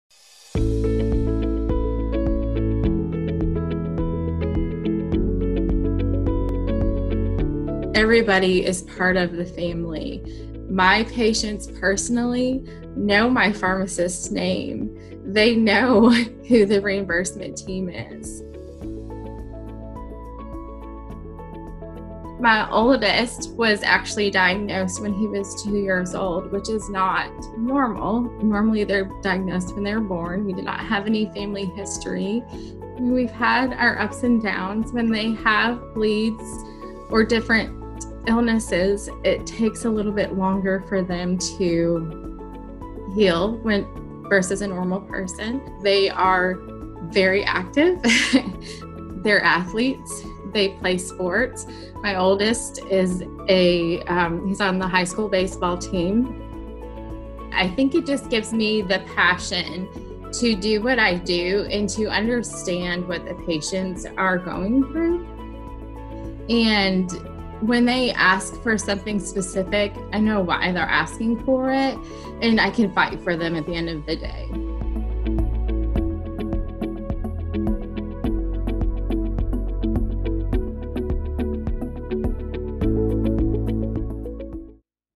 Meet the HPC Rep Shannon
Shannon is a Regional Hemophilia Specialist for HPC and shares how her personal story of living with two sons who have hemophilia inspires her and helps her serve other patients in the HPC community.